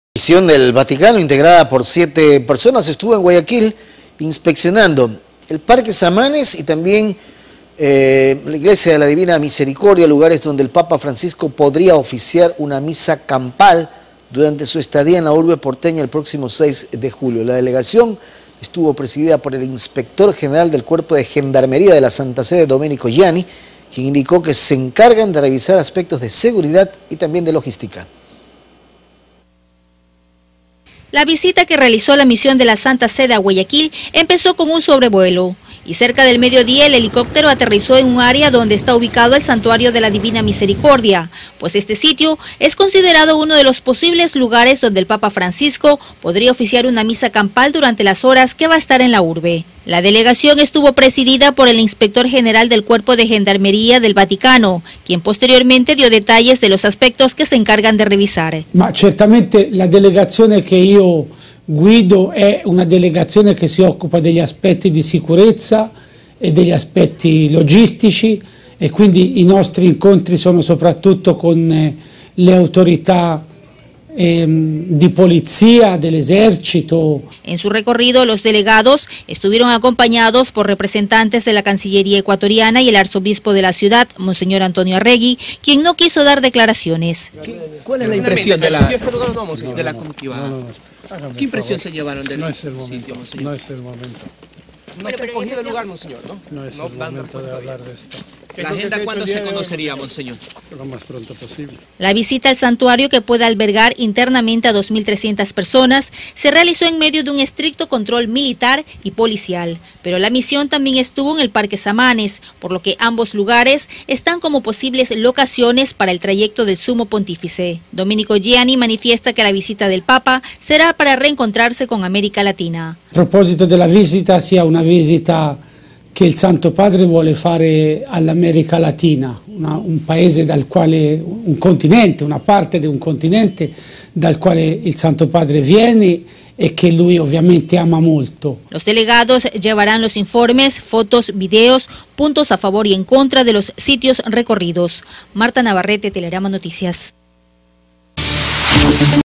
Misión del Vaticano inspeccionó lugares donde el Papa podría oficializar misa campal
Una misión del Vaticano integrada por 7 personas estuvo en Guayaquil inspeccionando el parque samanes y el santuario de la Divina Misericordia,  lugares donde el Papa Francisco podría oficiar una misa campal, durante su estadía en la urbe porteña el próximo 6 de julio.

La delegación, estuvo  presidida por el inspector general del cuerpo de gendarmería de la santa sede,  Doménico Giani, quien indico que se encargan de revisar aspectos de seguridad y logísticos…